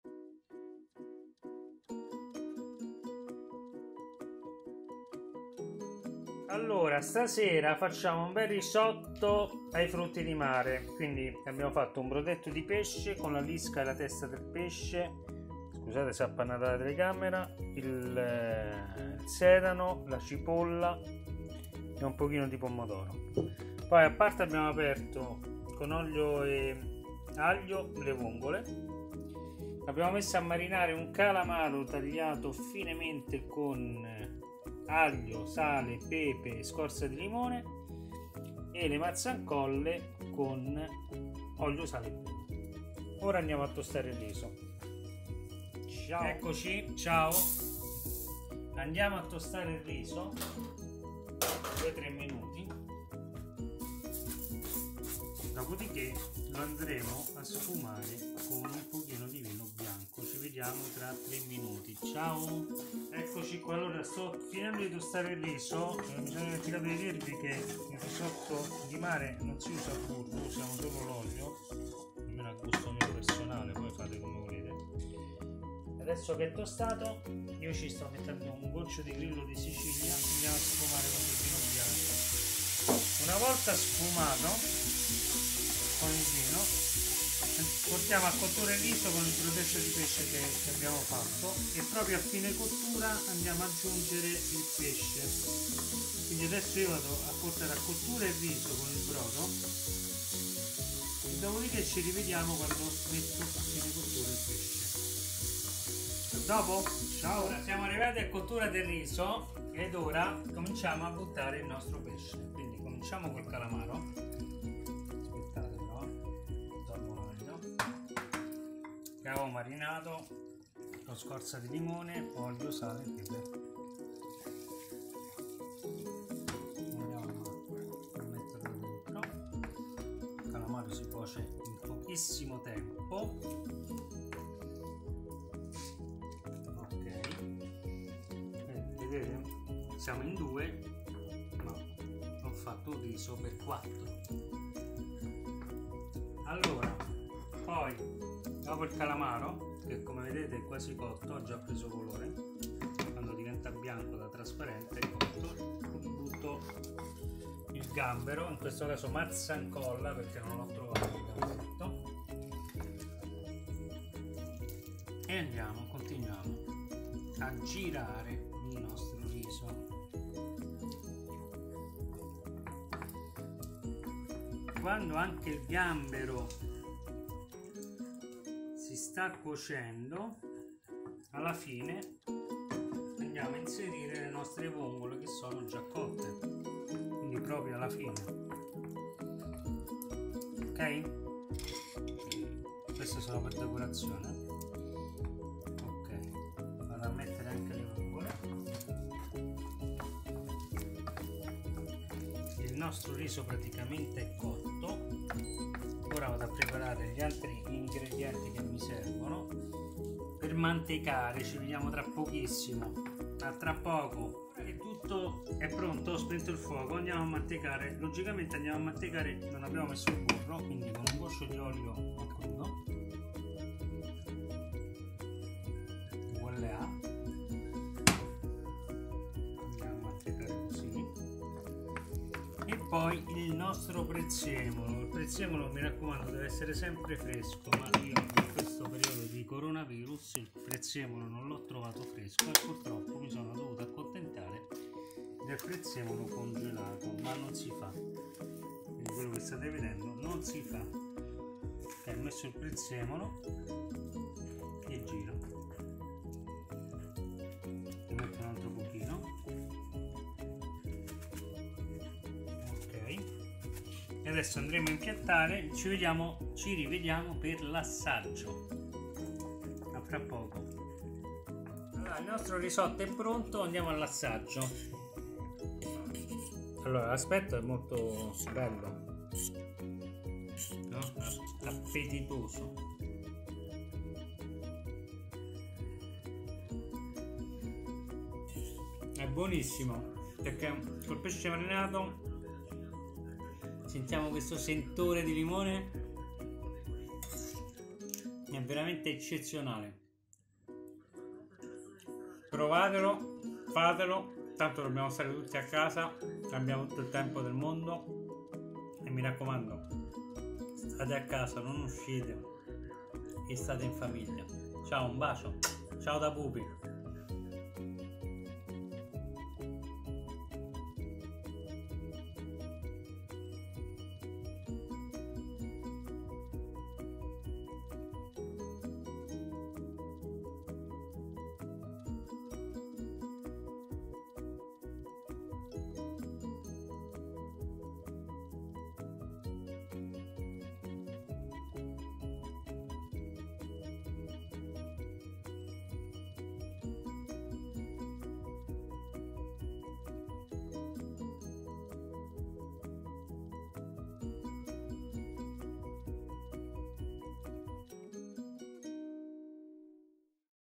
Risotto ai frutti di mare
Ciao a tutti da Pupi,
oggi vi propongo un buonissimo risottino ai frutti di mare, un piatto elaborato ma che vi darà delle grandi soddisfazioni, mi raccomando provatelo ed iscrivetevi al canale, un grande saluto e ricordatevi